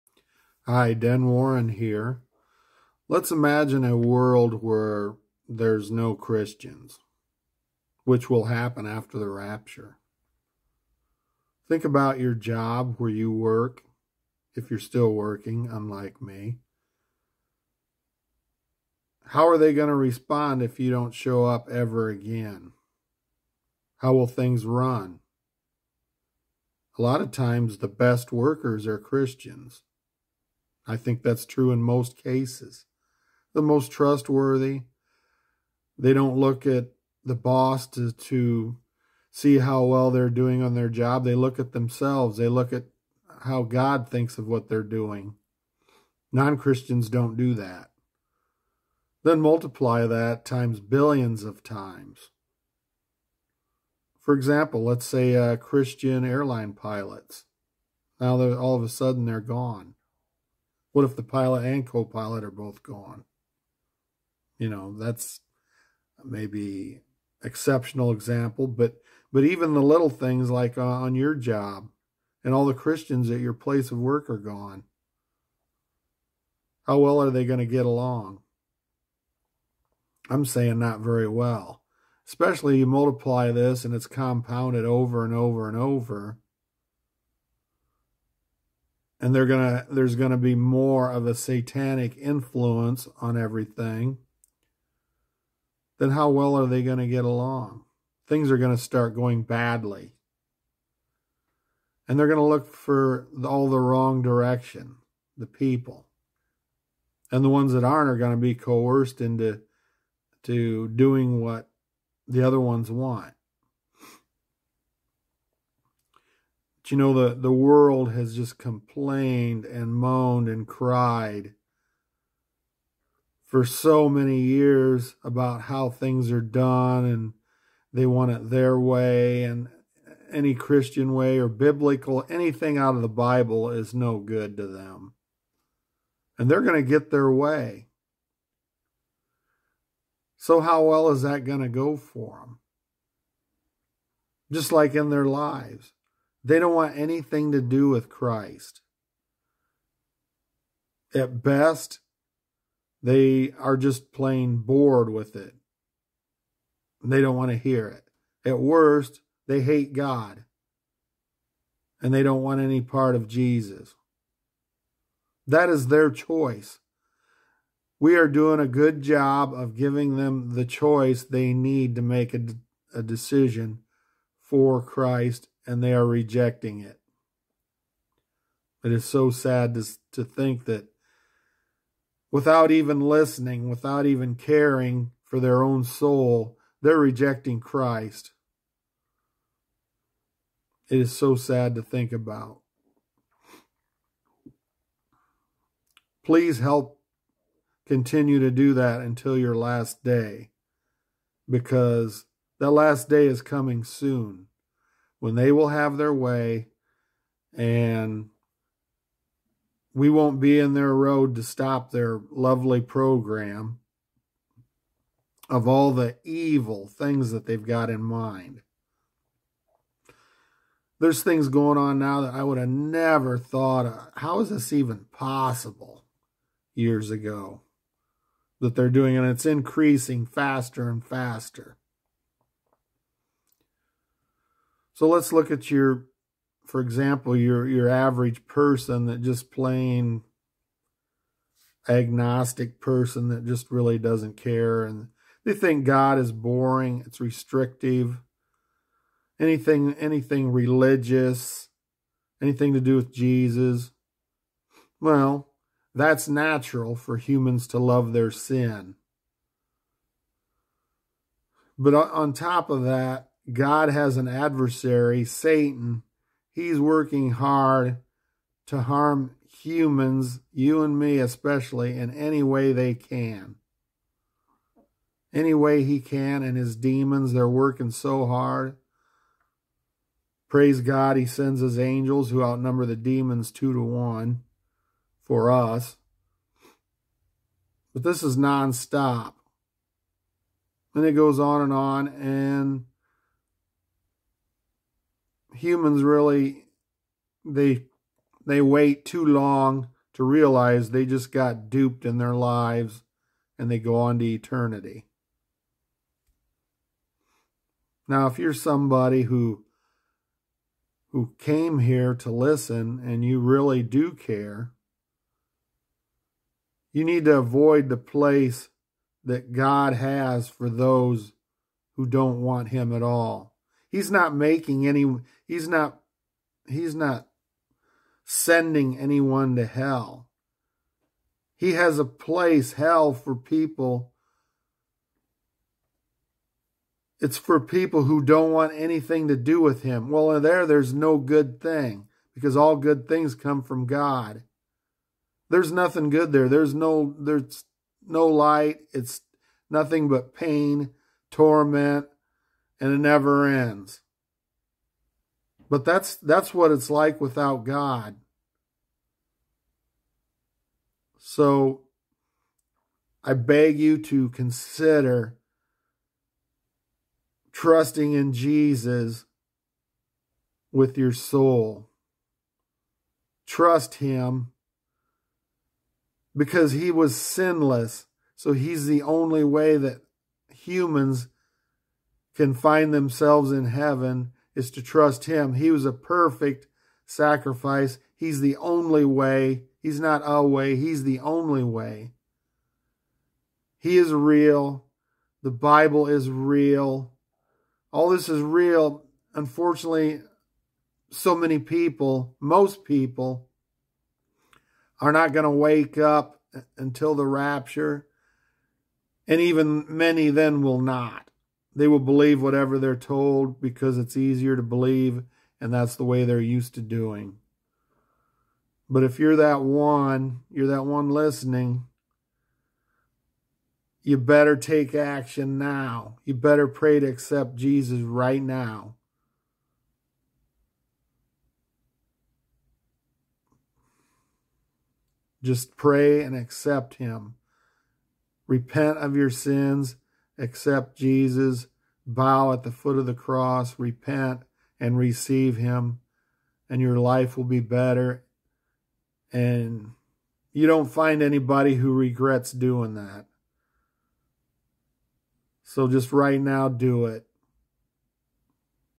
After the Rapture: World Without Christians
At long last the world will be rid of any Christian influence. How will it go?